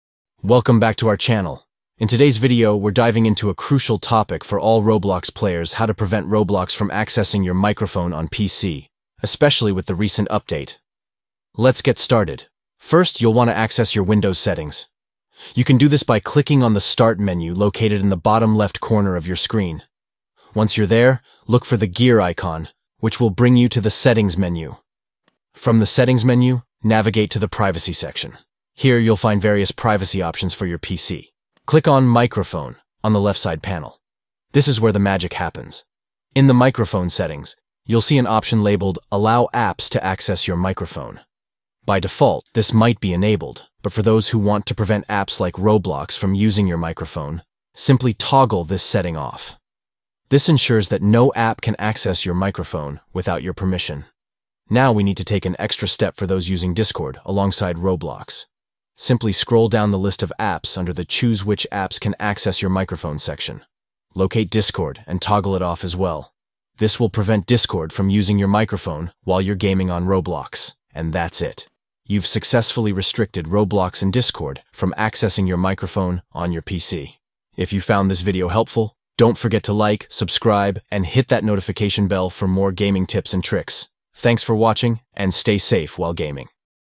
How to Prevent Roblox From Accessing Microphone On PC (New Update)
Learn how to prevent Roblox from accessing your microphone on your PC with the latest update. Keeping your privacy secure while gaming is essential, and in this video, we’ll guide you through simple steps to ensure your microphone remains off when using Roblox. Follow these easy steps to protect your audio privacy:
Steps:

1. Go into Windows Settings → Privacy → Microphone.
2. Disable "Allow apps to access your microphone."
3. Scroll down and disable the Discord app.

By following these steps, you can easily manage microphone access for Roblox and any other applications on your PC. Stay safe and game confidently.

Chapters:
00:00 Intro
00:12 Soultion
01:05 Outro